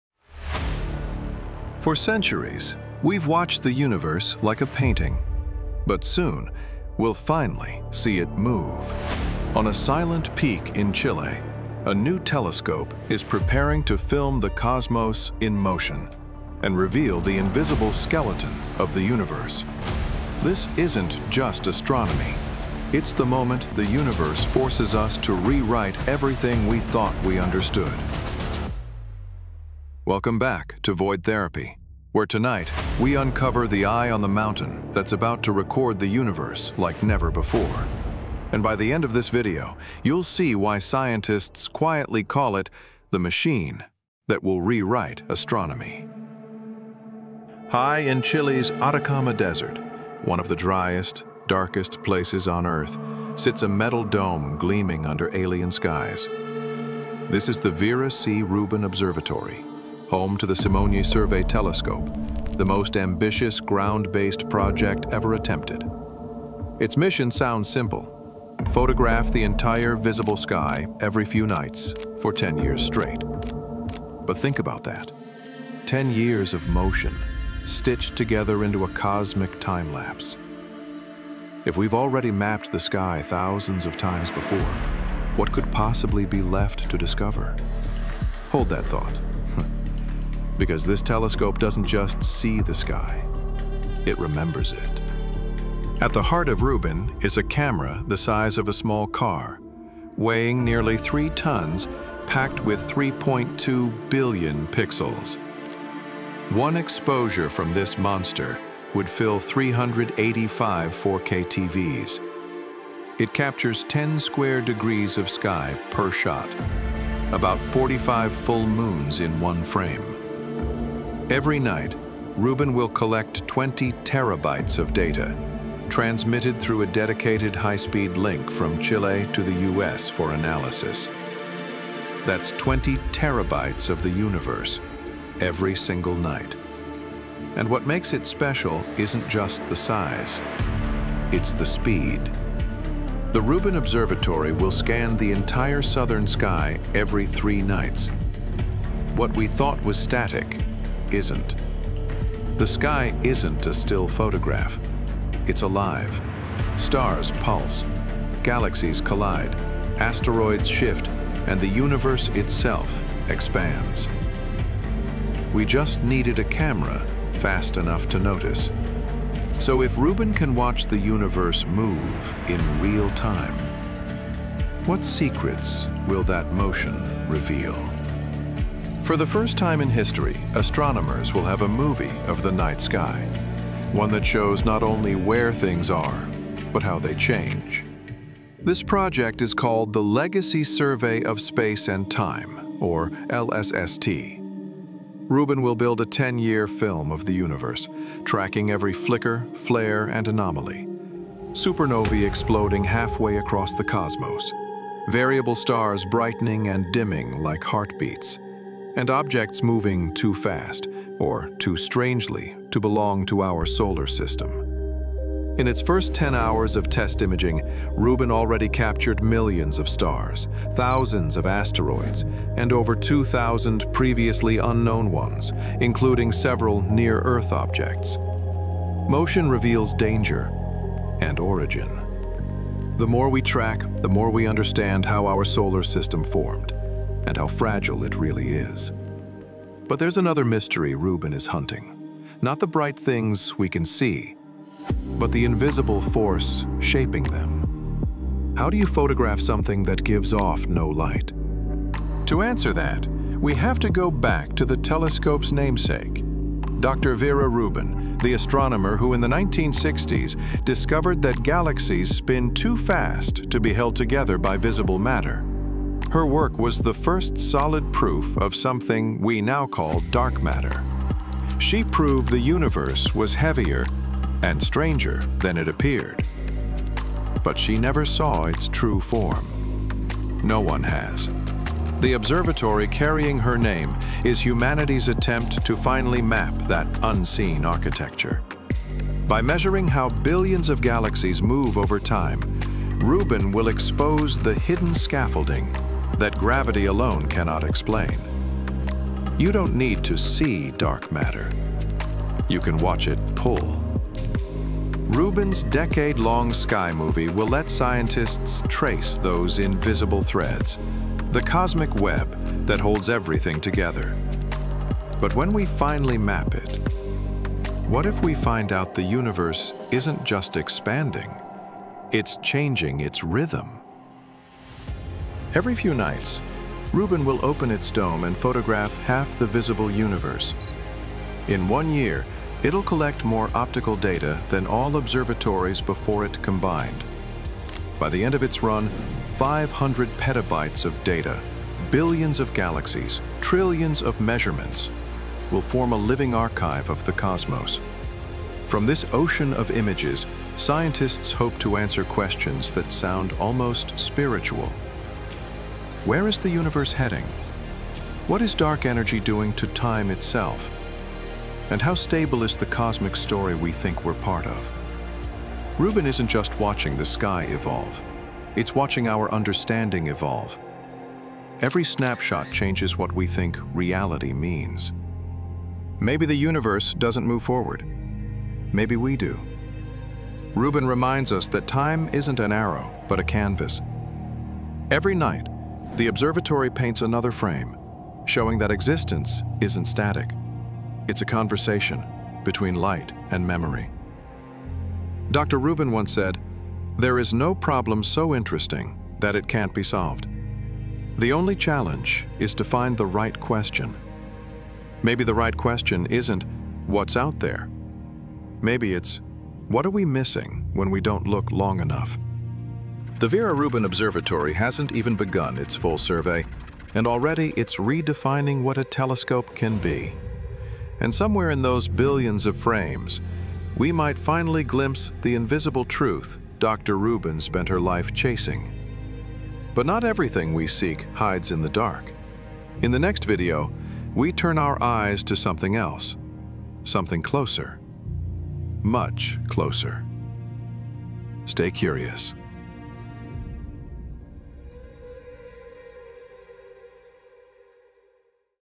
This New Telescope Will Change Everything
We're about to switch on a 3.2-billion-pixel eye in Chile, the Vera C. Rubin Observatory, a new telescope that will film the night sky in motion and reveal the hidden dark-matter skeleton of the universe.

In this Void Therapy deep dive, we explore how the LSST survey could rewrite astronomy, track dangerous asteroids, and change everything we think we know about space.

if you feel like supporting the channel, Check out my BMC

00:00 — Hook: The Sky That Remembers
00:22 — Intro: Welcome Back to Void Therapy
00:45 — Act I: The Eye on the Mountain
01:35 — Act II: The Camera That Sees Everything
02:25 — Act III: The Time-Lapse of the Cosmos
03:20 — Act IV: The Shadow of Vera Rubin
04:20 — Act V: The Universe in Motion
05:35 — Act VI: The Philosophy of Light
06:40 — Outro: The Legacy Begins | Stay Curious.

In case you have any video suggestions or wanna talk to me directly

Voice generation assisted by ElevenLabs

Topics explored in this video: the Vera C. Rubin Observatory in Chile’s Atacama Desert and its Simonyi Survey Telescope, the 3.2-billion-pixel LSST camera and its 10-year movie of the sky, how repeatedly imaging the entire southern sky reveals a dynamic, “living” universe, discovery and tracking of asteroids and near-Earth objects, supernovae and variable stars in motion, mapping the cosmic web and the influence of dark matter through galaxy movements, probing dark energy by watching the universe’s expansion over time, the legacy of Dr. Vera Rubin’s work on galaxy rotation, and how this telescope may quietly force us to rewrite what we think reality and “knowing the universe” actually mean.

Stay Curious.